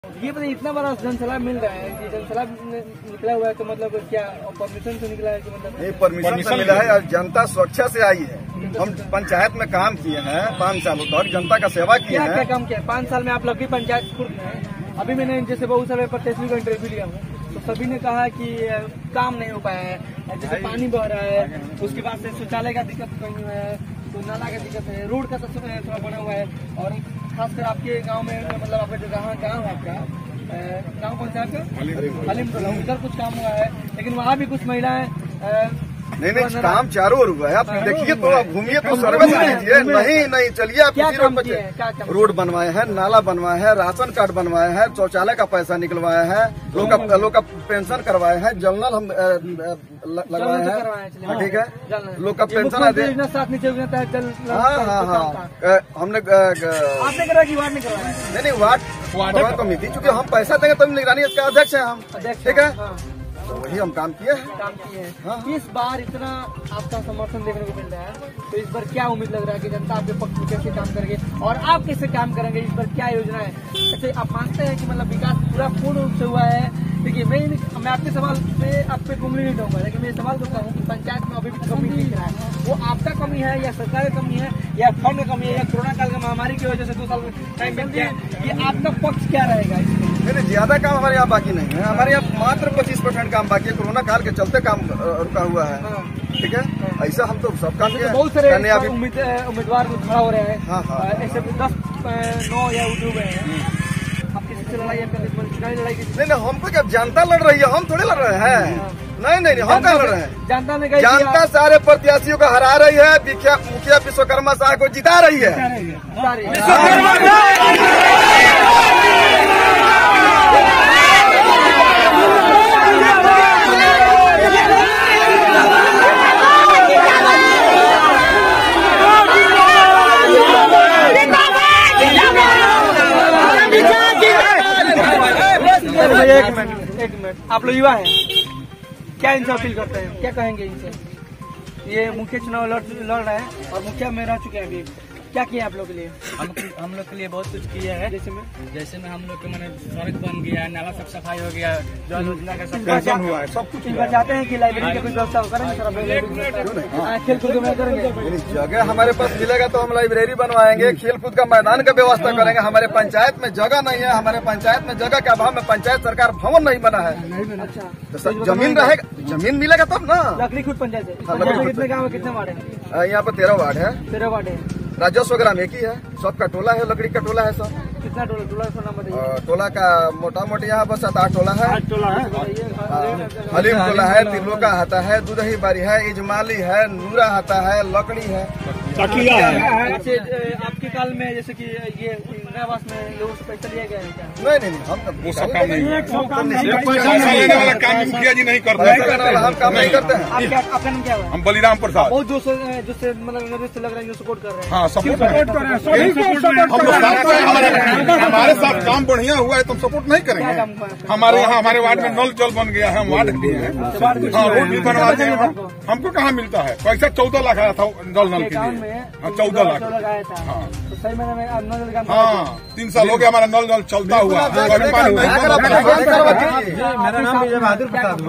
लकड़ी पंचायत से विश्वकर्मा साह का आज रैली निकला और गाँव के जनता सभी साथ मे है
लकड़ी पंचायत से विश्वकर्मा साह का आज रैली
निकला और  गाँव के जनता सभी साथ मे है